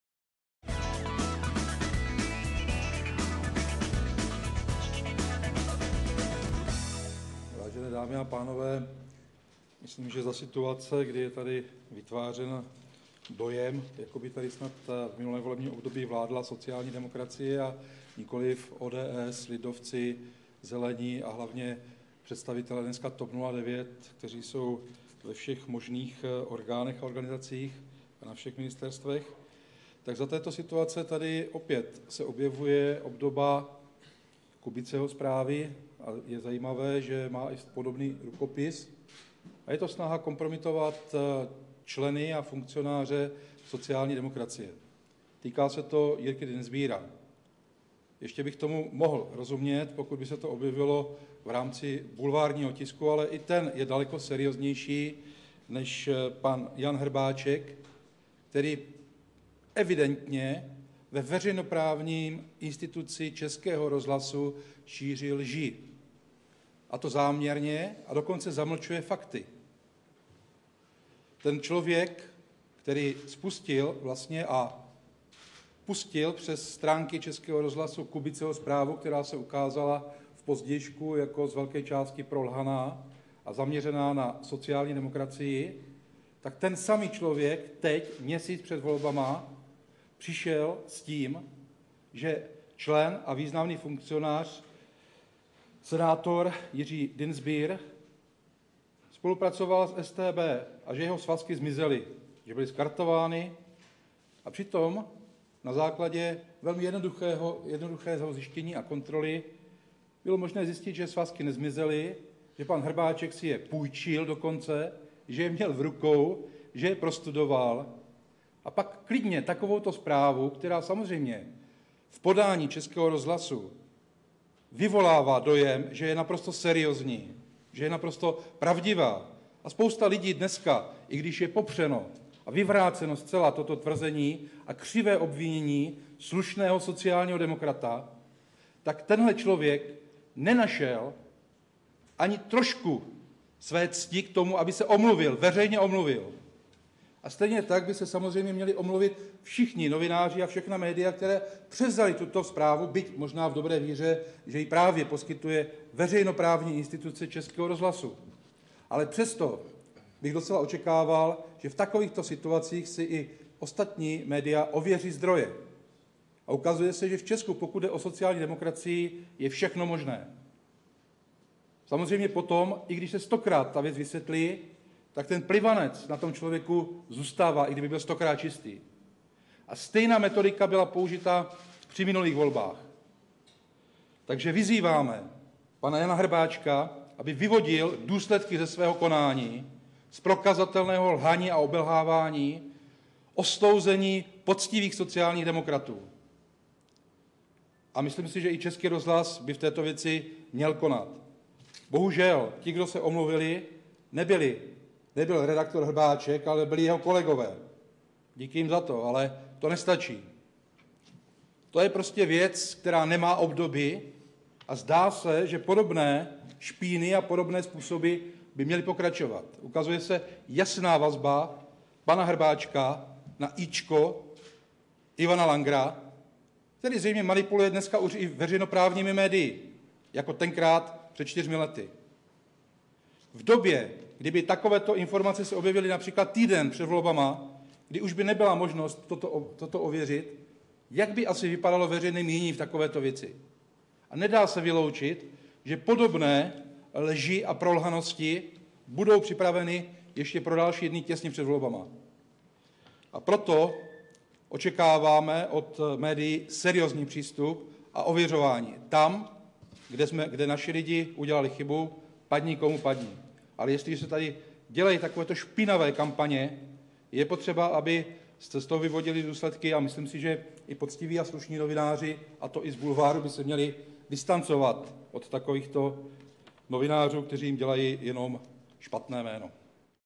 Zdeněk Škromach: Snaha kompromitovat Jiřího Dienstbiera
Zdeněk Škromach říká, že je zde vytváří dojem, jako by zde v minulém období nevládla ODS, ale ČSSD. Ve vládě byla ale ODS s KDU a SZ a lidé, kteří jsou nyní v TOP 09. Nyní se zde opět objevuje cosi jako Kubiceho zpráva, která se nyní týká Jiřího Dienstbiera.
Redaktor Českého rozhlasu Jan Hrbáček vypustil takovouto zprávu v Českém rozhlasu o tom, že Jiří Dienstbier spolupracoval s StB a že jeho svazky zmizely a byly skartovány. Svazky nezmizely a pan Hrbáček je měl dokonce zapůjčené a pak vypustí takovouto zprávu, která je pak jako zpráva ČR brána za seriózní. Když je pak toto tvrzení zcela vyvráceno, nenašel tento člověk kousek cti k tomu, aby se veřejně omluvil.

Pokud jde o sociální demokracii, pak je v Česku možné všechno a pokud se pak vše vysvětlí, plivanec již na osočeném člověku zůstane. I na tomto případu byla použita stejná metodika špinění jako při minulých volbách.